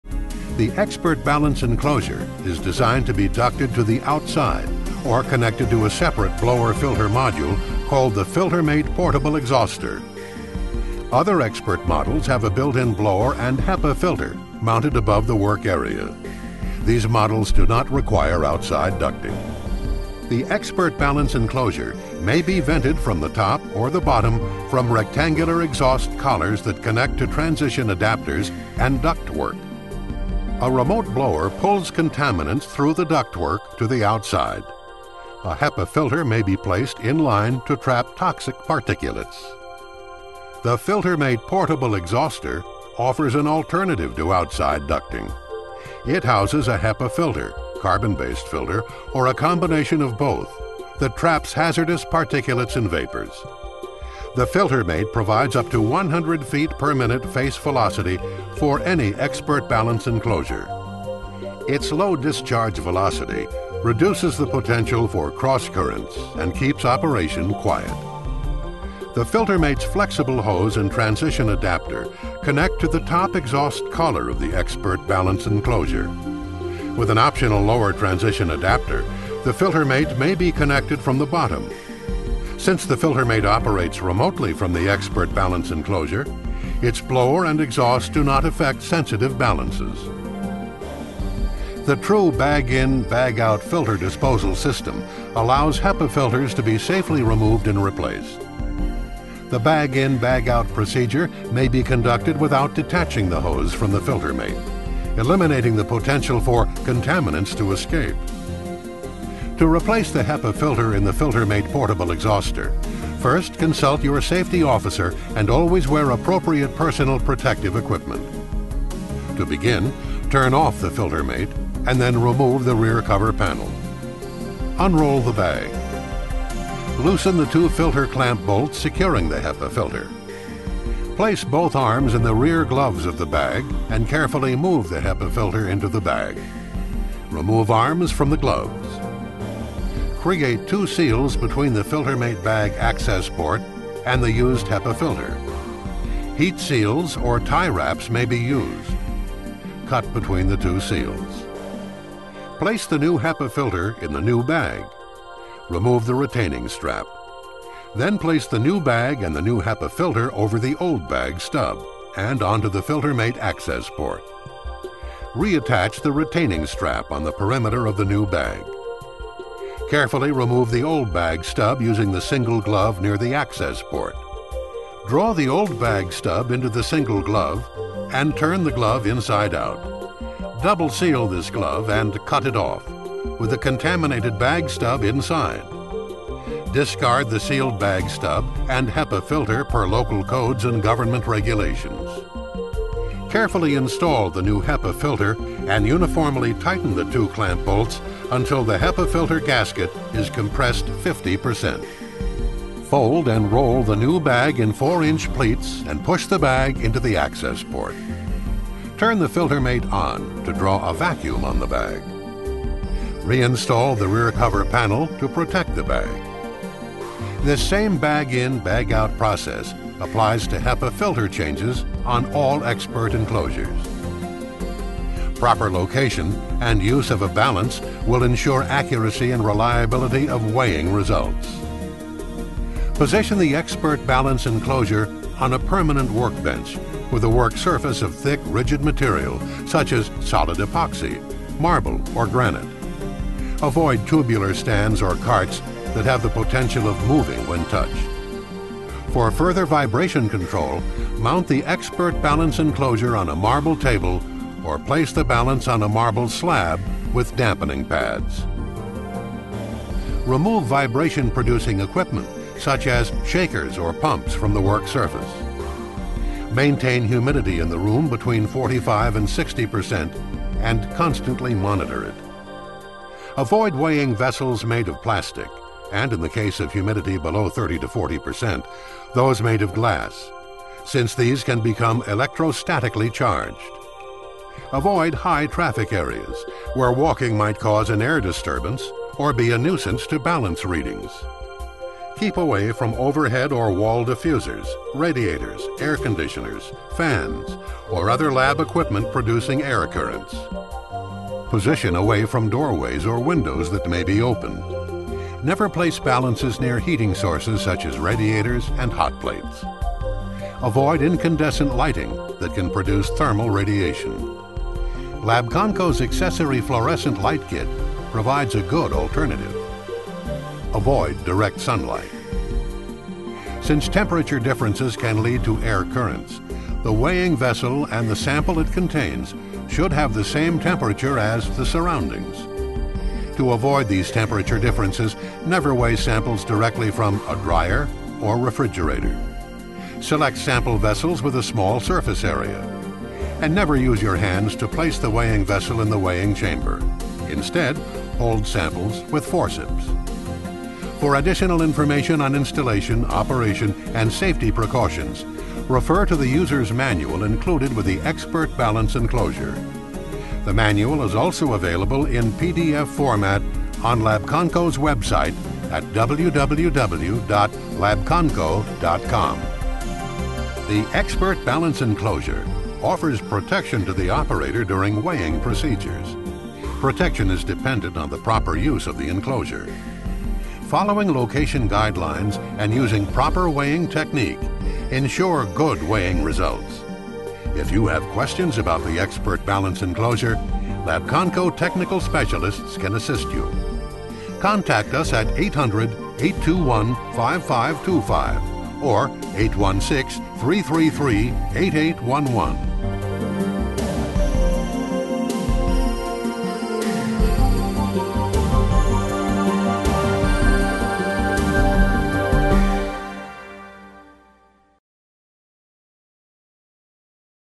Labconco XPert Balance Enclosures Part 2 (2004)
Part 2 of video demonstrating the features, benefits and proper use of Labconco's XPert Balance Enclosures.